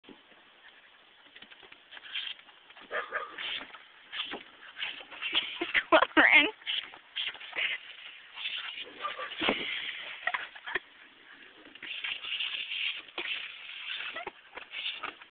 scottie water fight!
Attack of the fierce water hose...or is the dog the one attacking?!! Who will win...the dog or the hose??